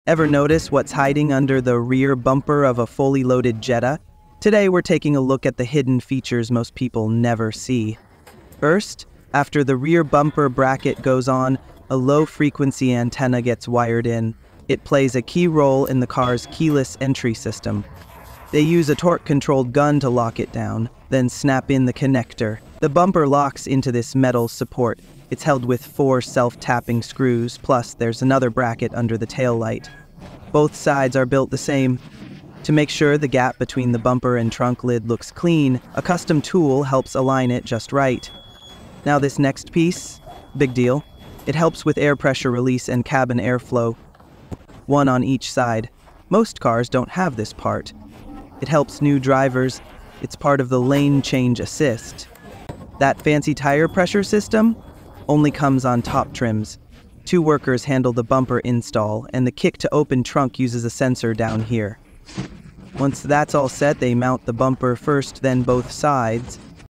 What’s Hiding Under the Rear Bumper of the Fully Loaded VW Jetta
Ever wondered what’s really hiding under the rear bumper of a fully loaded Volkswagen Jetta? In this video, we take you behind the scenes at the factory to reveal the hidden features that most drivers never see. From keyless entry antennas to active safety sensors and premium tech like kick-to-open trunk sensors and lane change assist—this isn’t your average bumper setup. Whether you're a car enthusiast or just curious about what sets top-trim models apart, this deep dive has you covered.